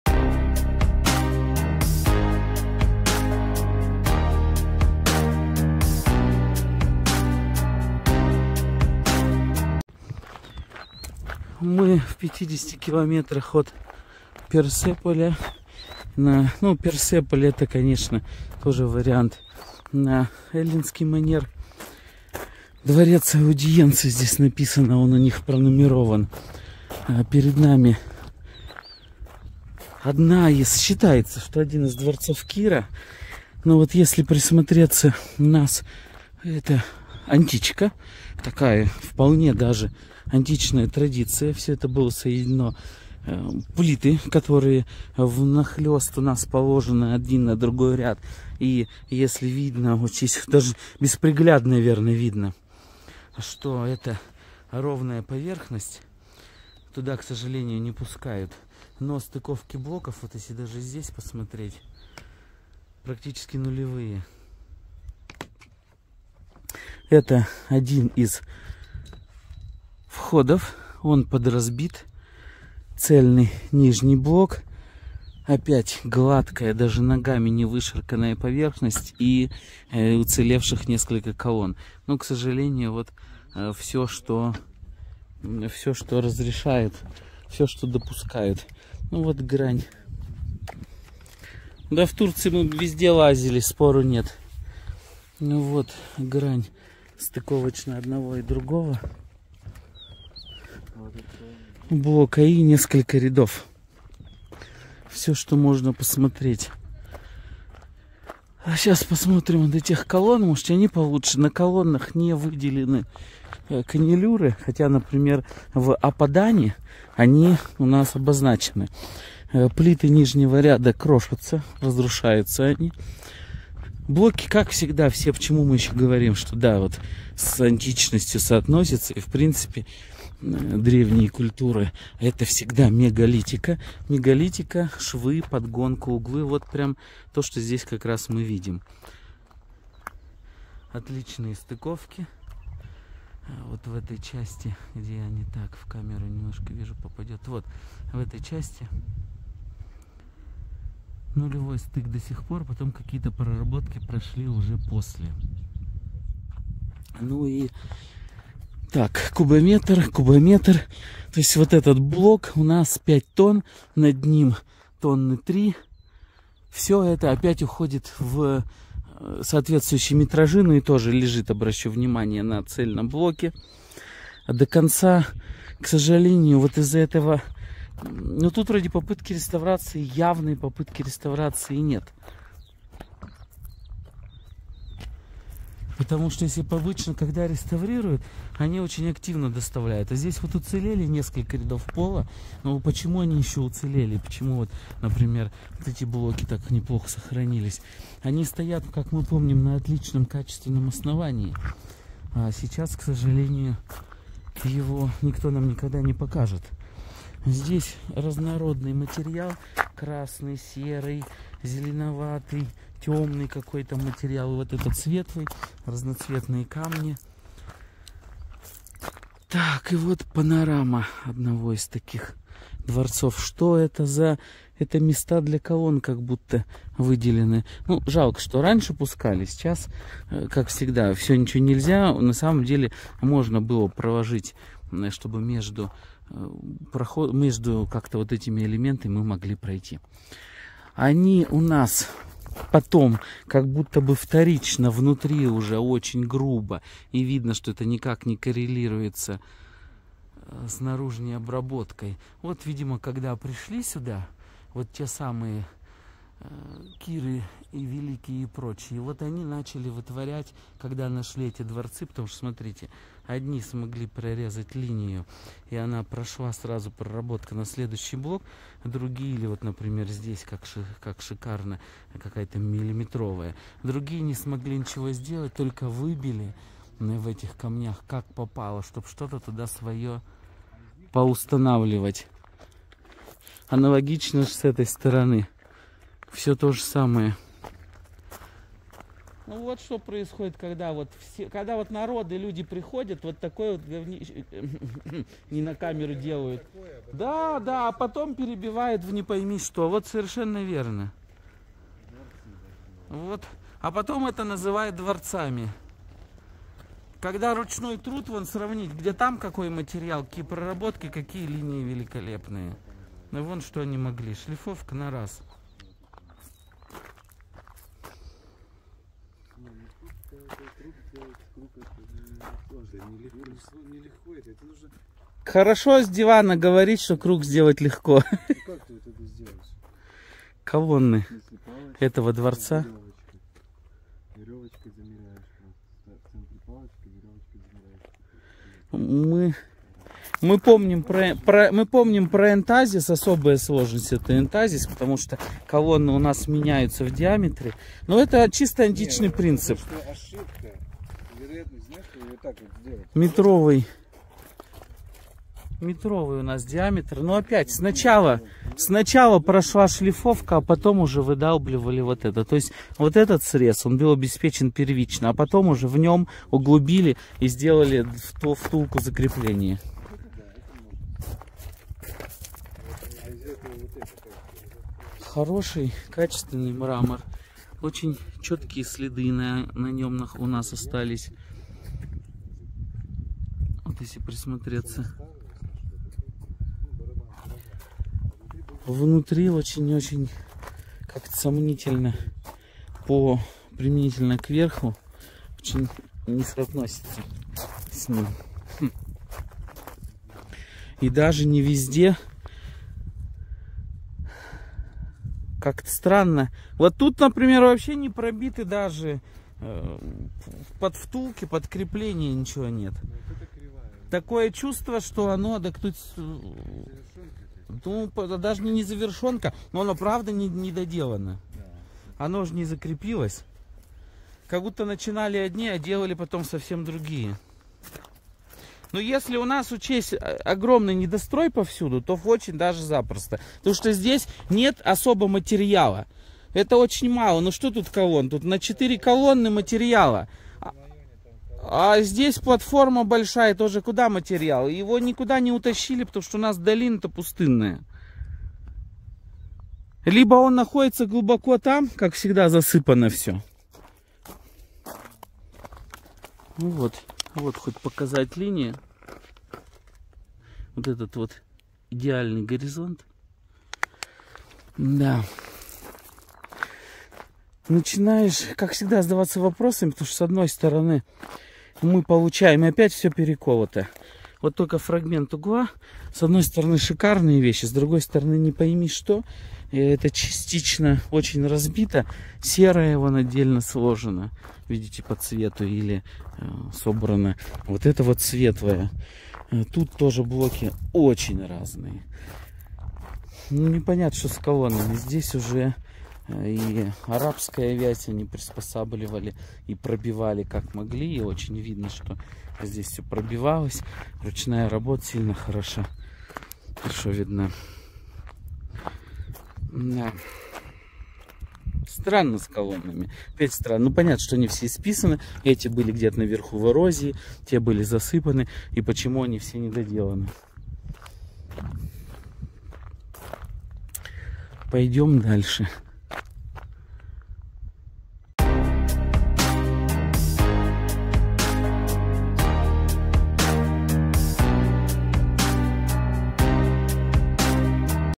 Пасаргады, Иран: идеальные каменные сооружения существовали‼️
📚🏛📚ПРИОБРЕТИТЕ КНИГИ, ПОСЕТИТЕ САЙТ ПРОЕКТА